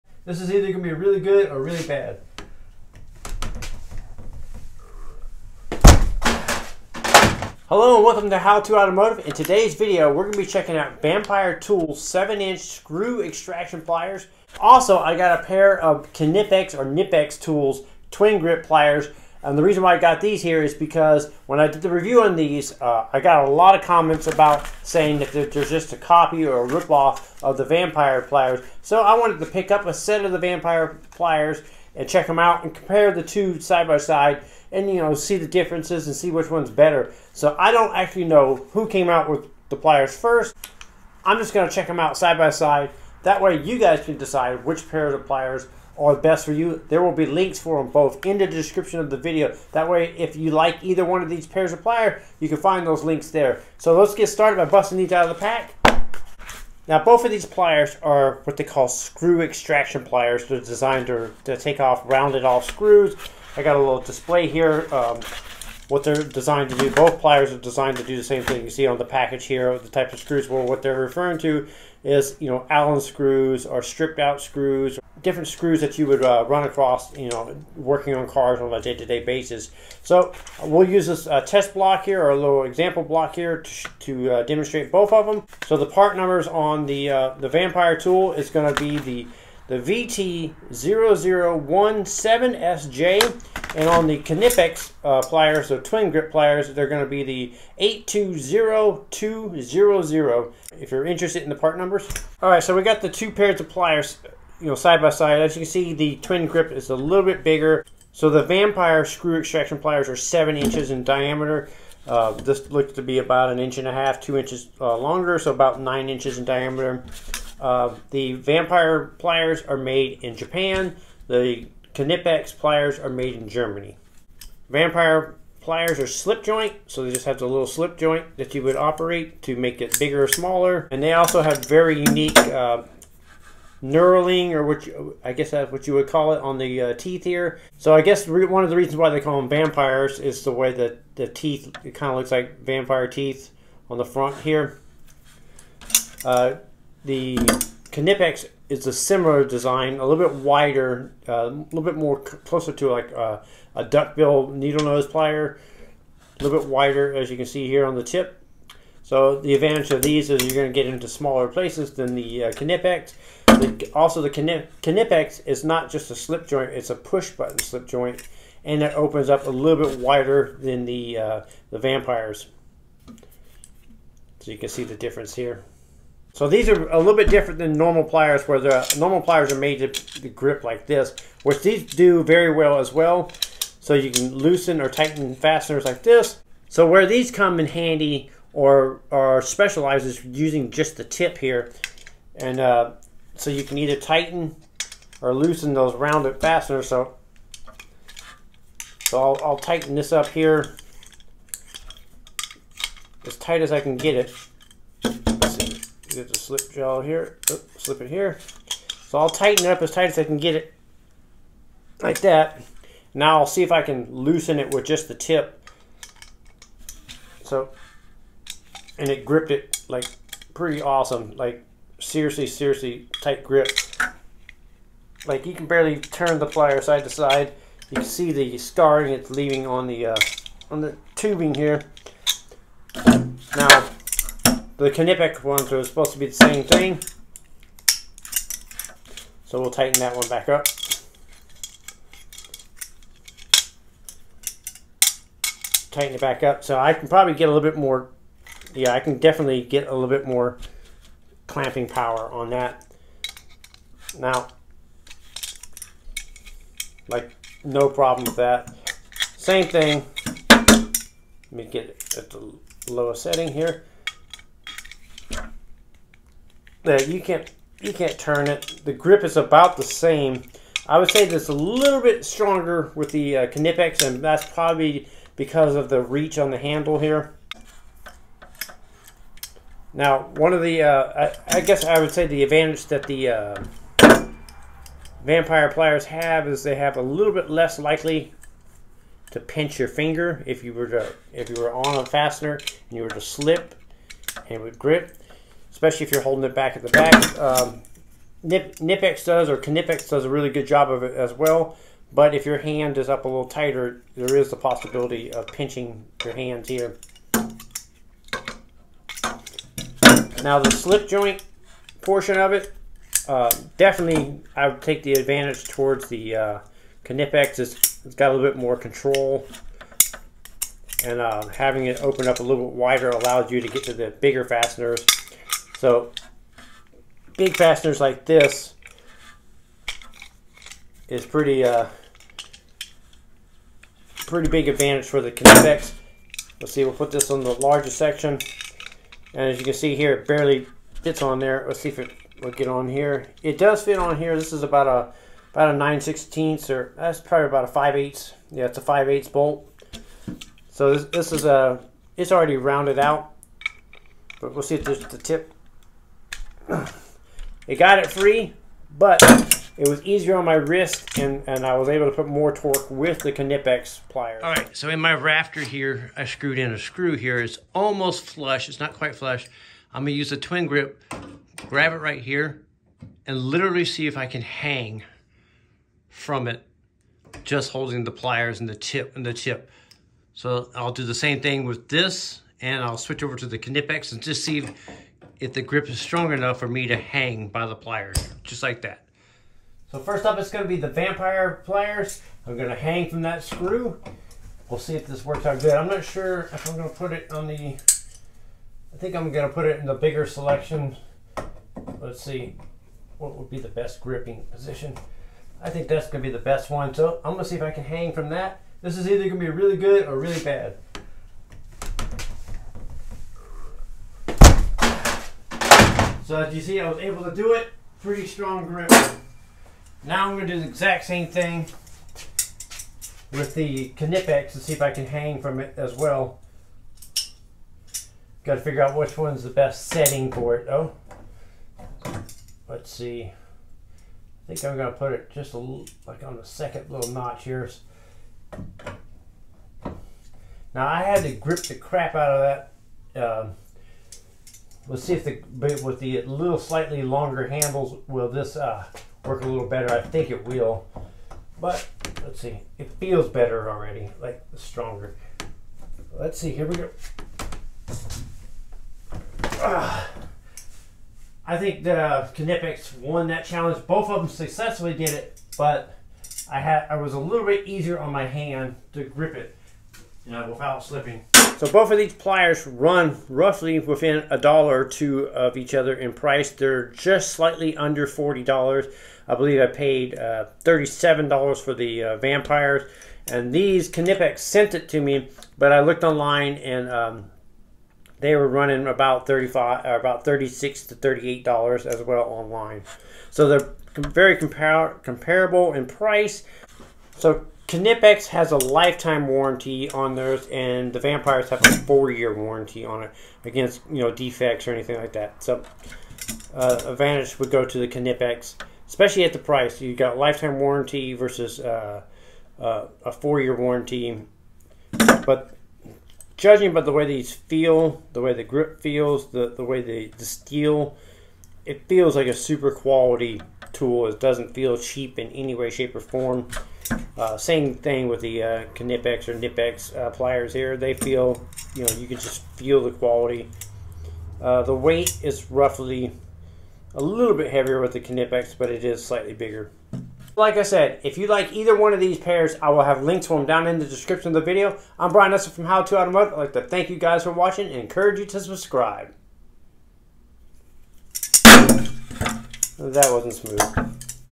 Are Japanese VAMPLIERS Better Than German Knipex Pliers?
Brian Eslick from How to Automotive put the brand new Knipex Twin Grip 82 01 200 and Vampliers VT-001-7SJ. Slip Joint Screw Extraction
pliers to work so you can tell witch ones are best for you.

New! How to Automotive T-shirts, stickers, and more!